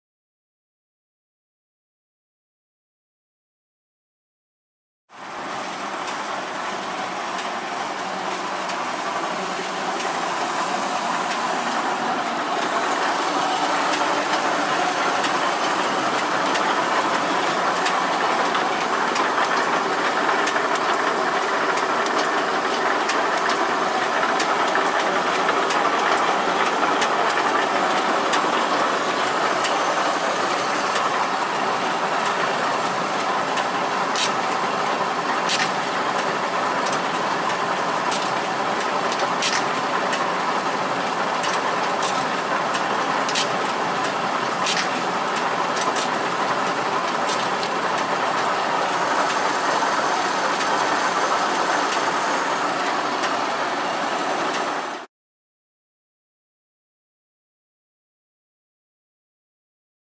Arenco Model AM2000 Tube Filler Part 2 SIGMA Equipment
This is a functional video of a Arenco Model AM2000 Tube Filler. for more available equipment.